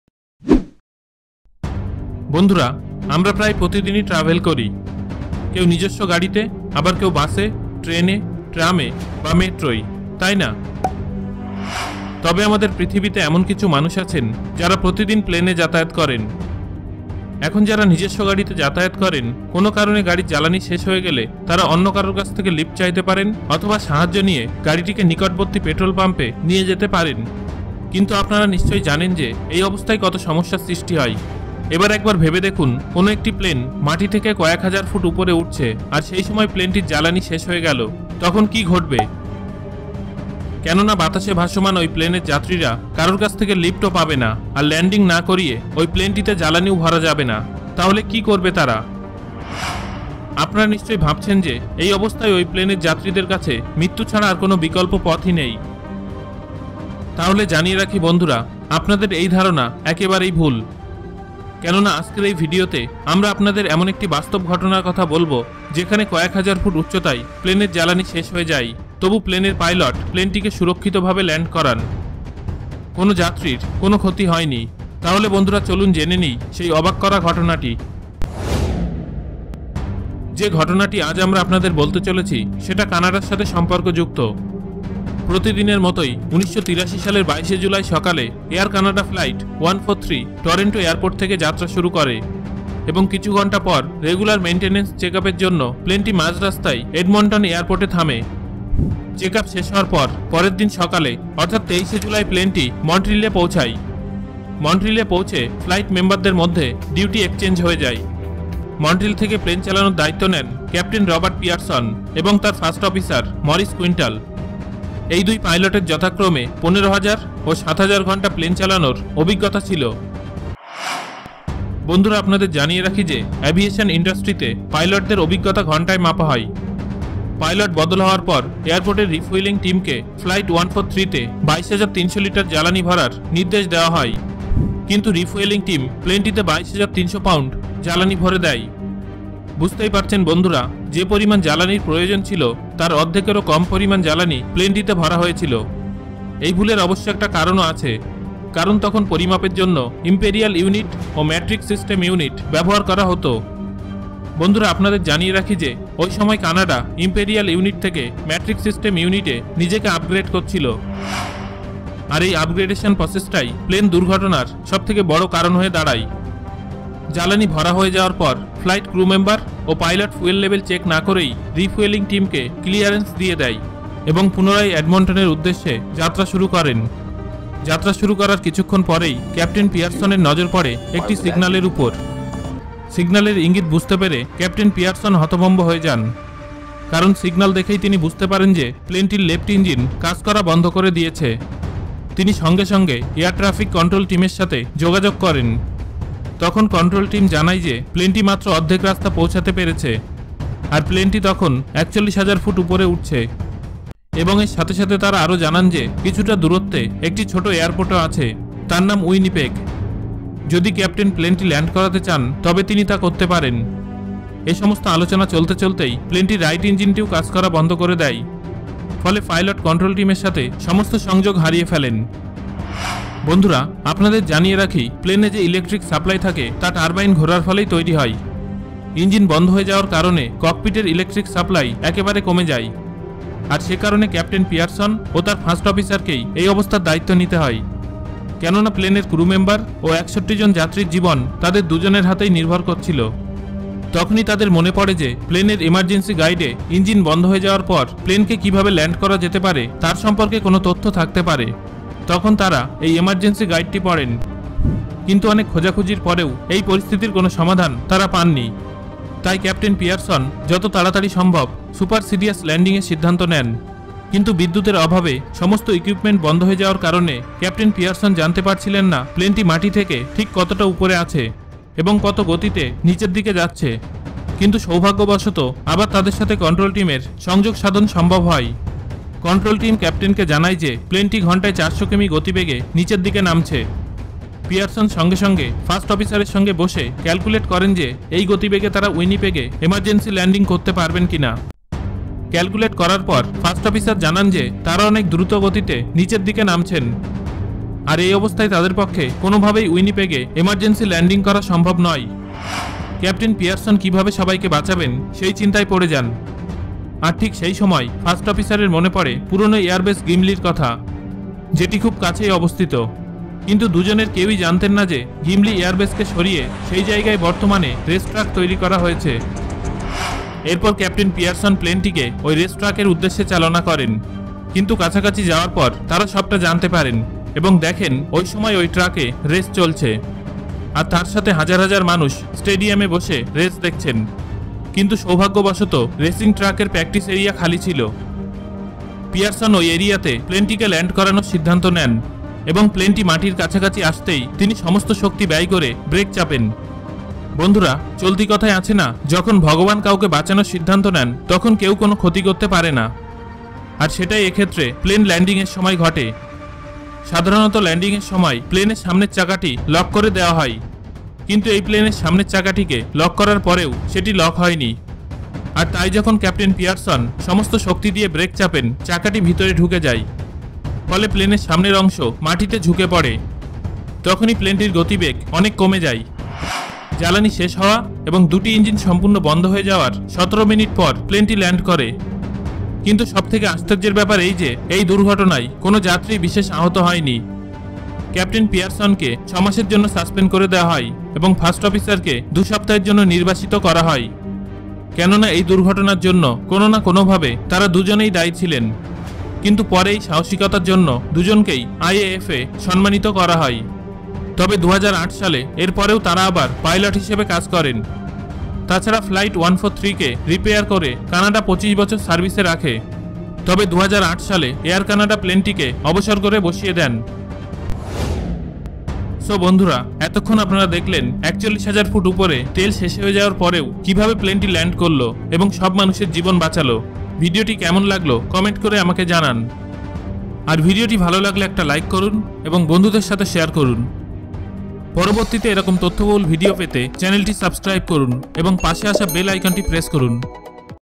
৪১,০০০ ফুট উপরে প্লেনের তেল শেষ- তারপর কি হলো | An Airplane ran Out of Fuel at 41,000 Feet | Gimli
Unbelievable Story of Flight 143 when Fuel Tank was Empty at 41000 Feet in Bangla:

Air Canada Flight 143 was a Canadian scheduled domestic passenger flight between Montreal and Edmonton that ran out of fuel on July 23, 1983, at an altitude of 41,000 feet midway through the flight. The crew was able to glide the Boeing 767 aircraft safely to an emergency landing at a former Royal Canadian Air Force base in Gimli, Manitoba that had been turned into a motor racing track. This unusual aviation incident earned the aircraft the nickname "Gimli Glider".